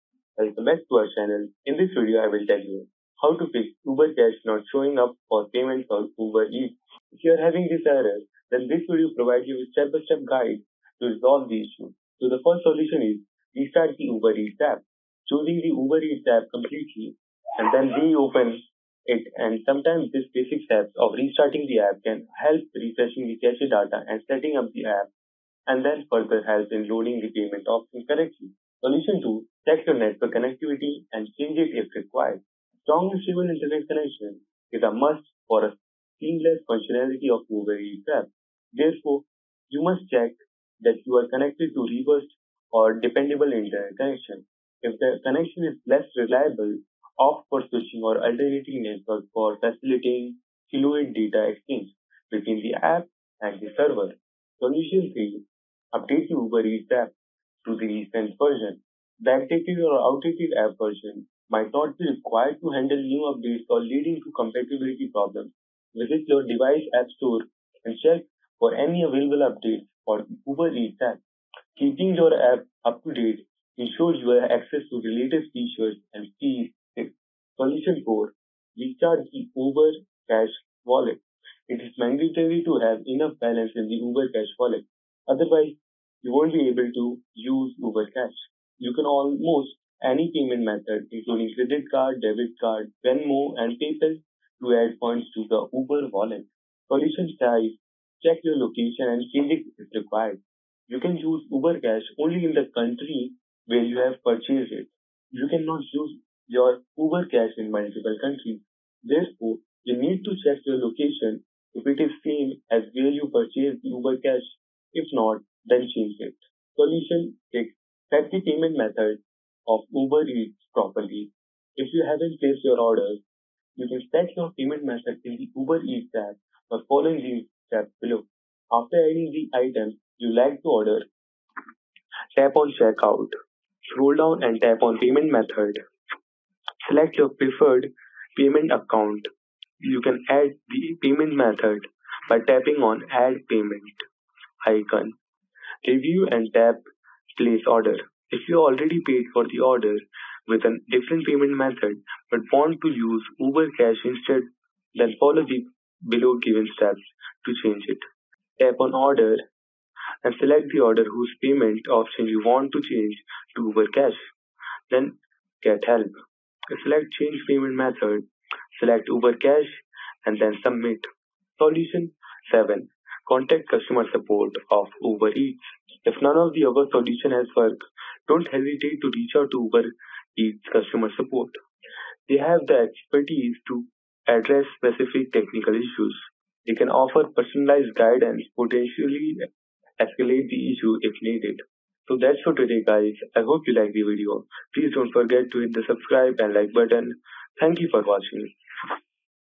How To Fix Uber Cash Not Showing Up For Payment On Uber Eats
Is your Uber Cash not appearing as a payment option on Uber Eats? Don't worry, we've got the solutions you need to get it working again. In this comprehensive guide, we'll walk you through step-by-step troubleshooting to resolve the issue. From restarting the Uber Eats app and checking network connectivity to updating the app and restarting the Uber Cash wallet, we cover it all. Make sure you're in the right location, set up your payment method properly, and if all else fails, don't hesitate to contact customer support. Say goodbye to payment issues and enjoy your meals hassle-free with Uber Eats. 🍔🛵💰

Chapters:
00:00 Introduction
00:11 Solution 1: Restart the Uber Eats App
00:27 Solution 2: Check Your Network Connectivity
00:53 Solution 3: Update the Uber Eats App
01:15 Solution 4: Restart the Uber Cash Wallet
01:35 Solution 5: Check Your Location
01:55 Solution 6: Set the Payment Method Properly
02:50 Solution 7: Contact Customer Support
03:10 Conclusion